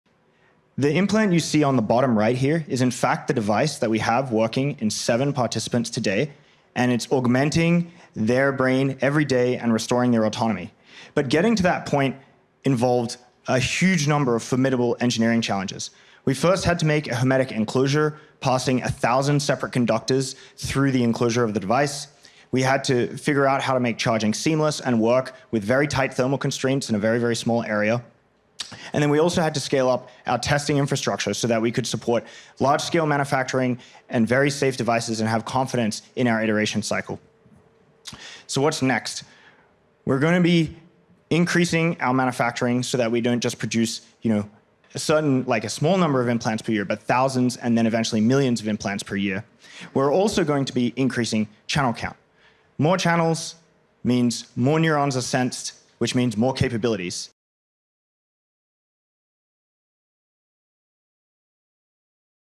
Brain Implant Challenges: Engineering, Manufacturing, and the Future
Join us as we delve into the cutting-edge world of implant development! We explore the remarkable engineering challenges faced in creating life-changing devices, showcasing our advancements and future plans for scaled manufacturing and increased channel counts, paving the way for enhanced neuron sensing and greater capabilities.